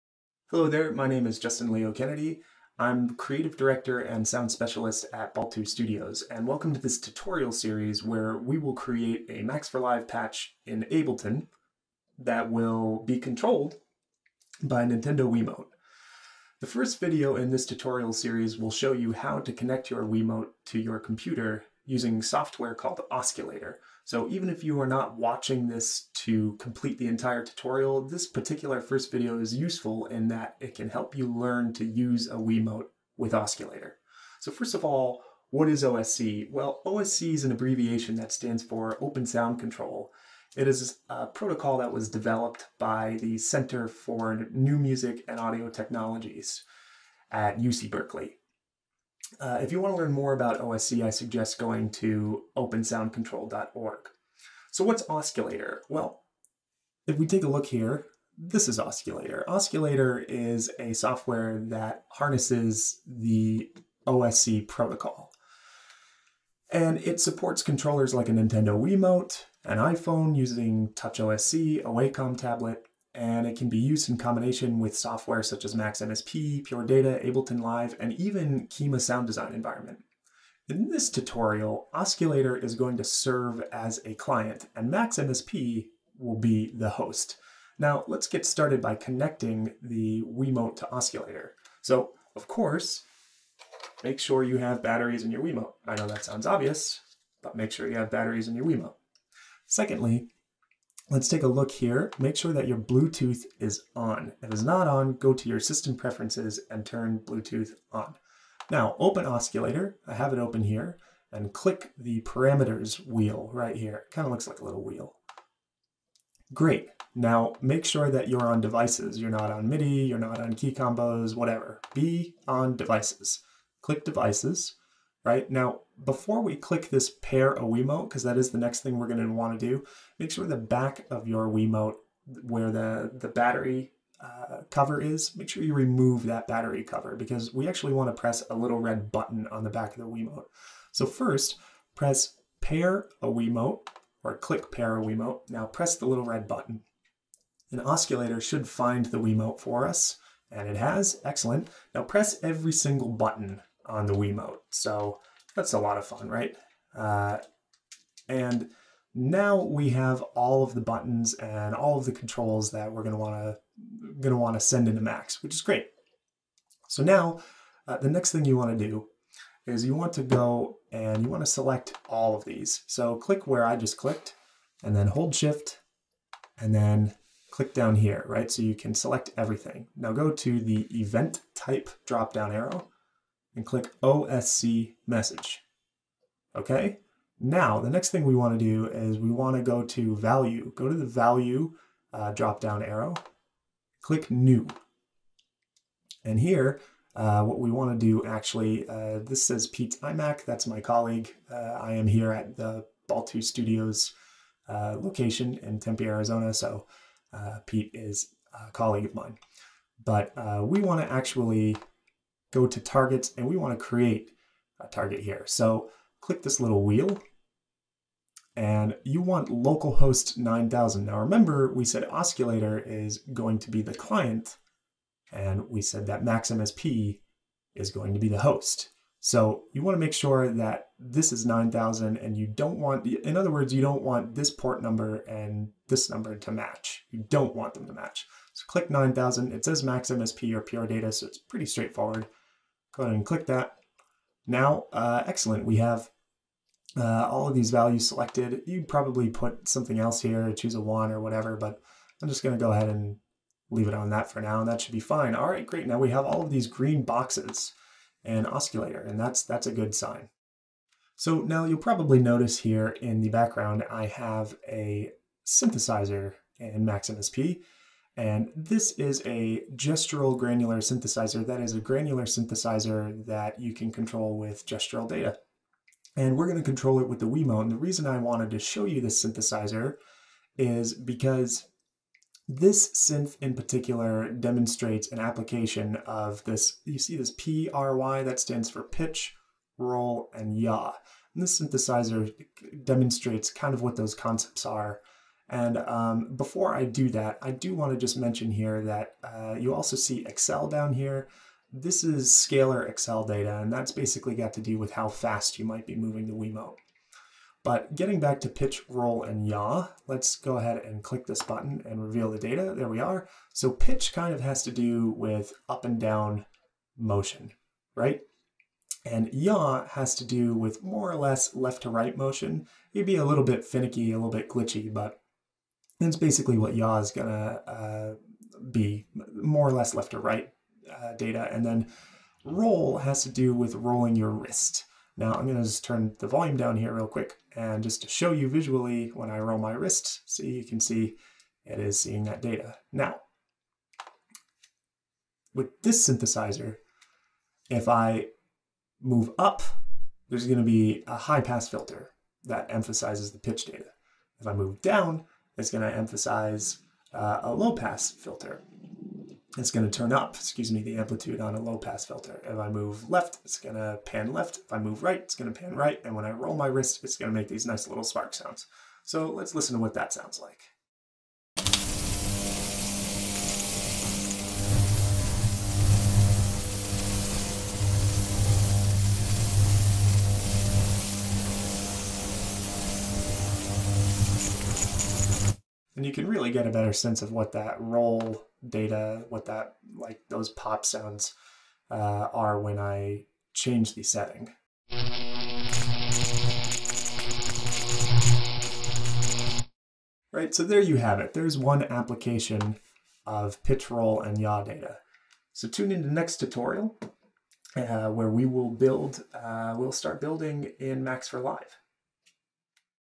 Baltu Tutorial 1 (part 1): Set up OSCulator & Wiimote / PRY Explanation
Baltu Studios' inaugural tutorial, part 1. This 5-part tutorial series will teach you how to create and control an instrument in Ableton Live using a Nintendo Wiimote, via Max for Live and OSCulator.